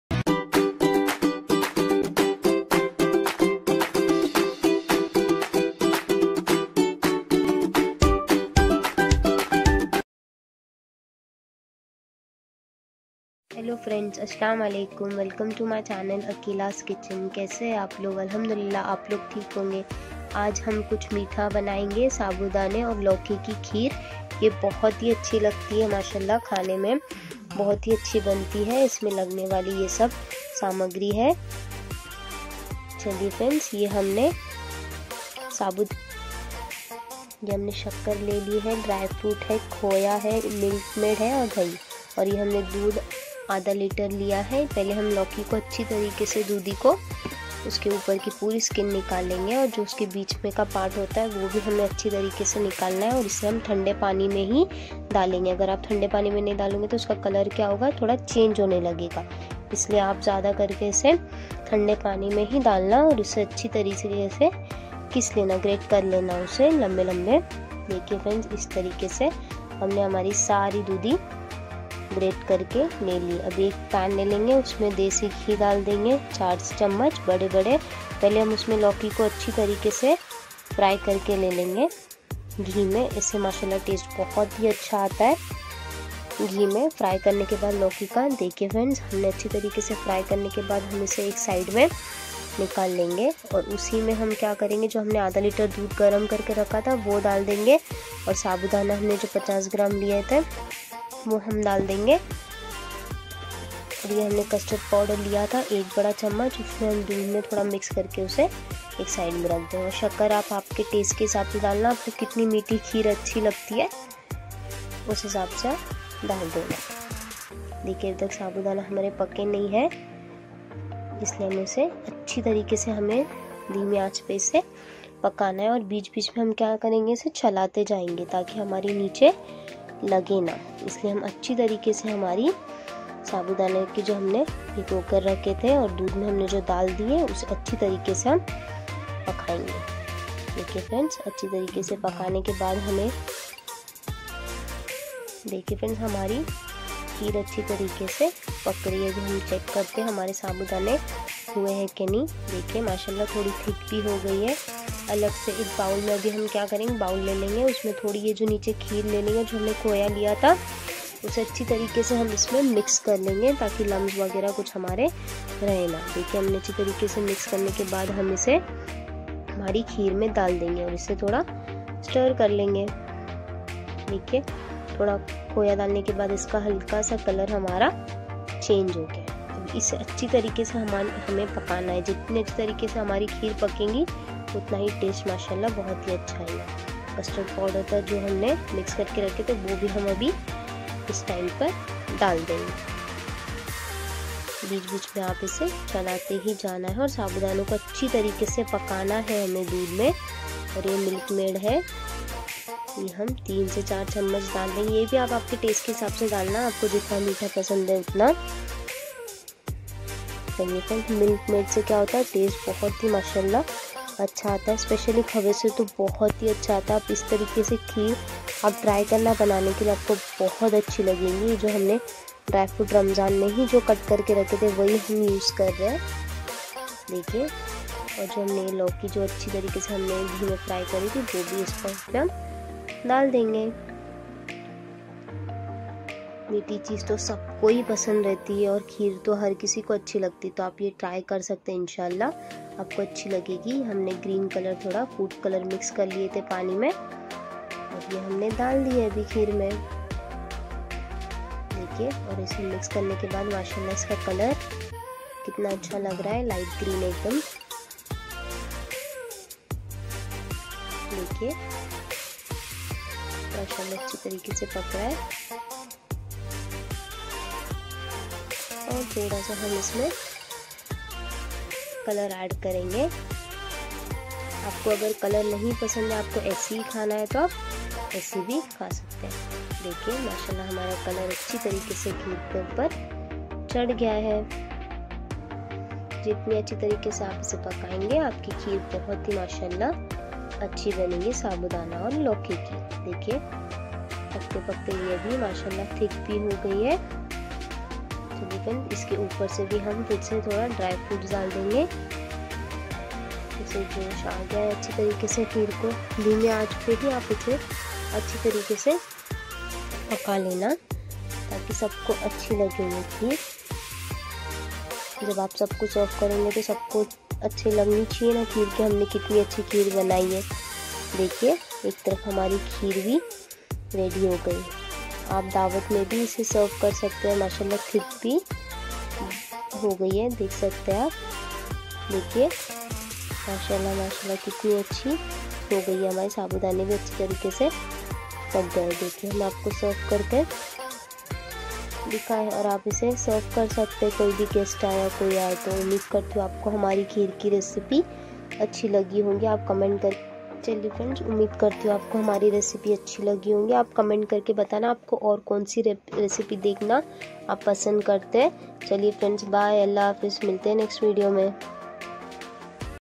Lauki & Sabudana Ki Kheer | Sweet Indian Dessert | Kheer Recipe
Lauki ya Kadu ki Kheer Garmyion kay Liye Perfect Recipe in Hindi

Ye hai meri lauki ki kheer ki recipe. Umeed hai aapko pasand ayegi!